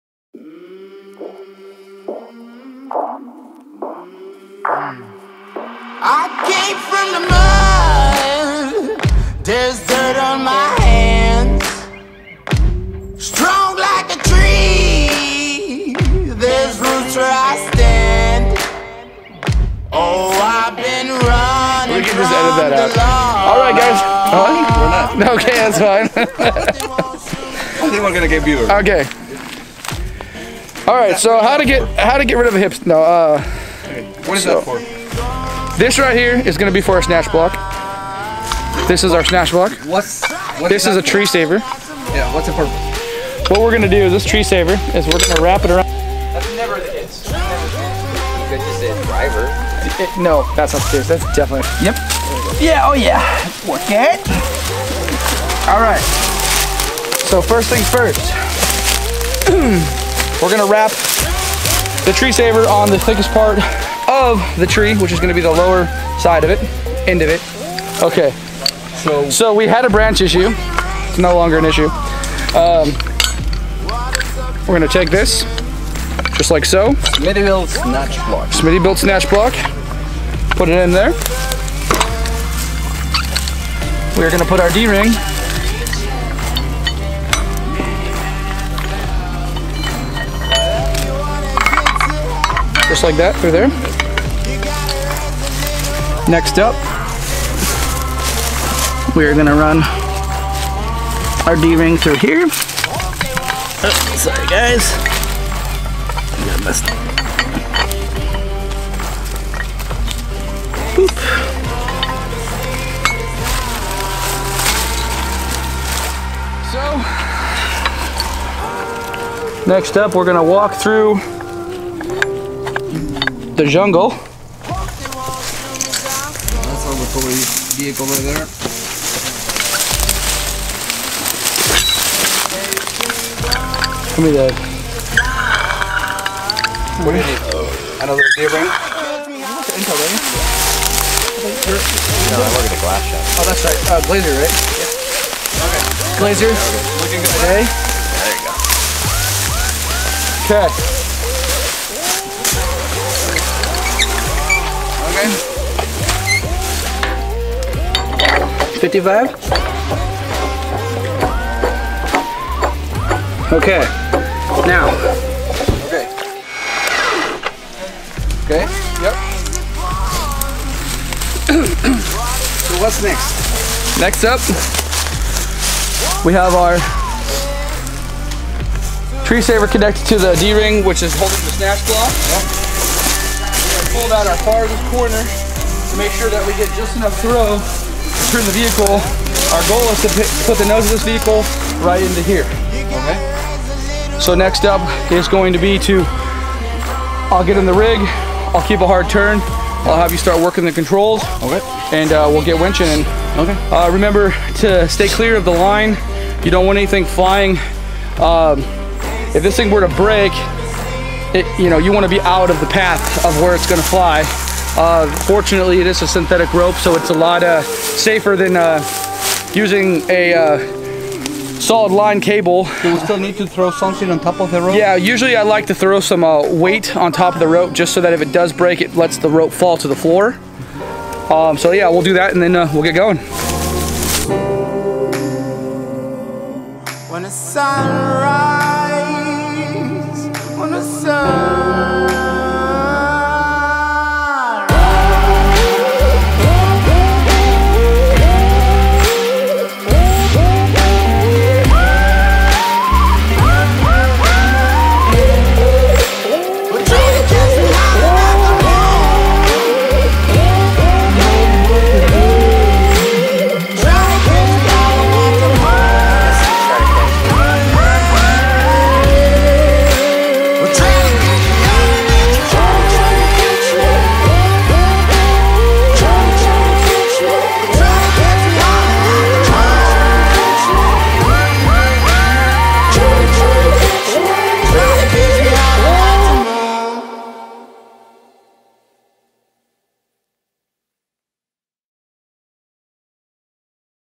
Example of use of a single Snatch Block
Adam demonstrates in this short clip the proper way to use a single snatch block to recover a vehicle with a winch at a 90 degree angle extraction point.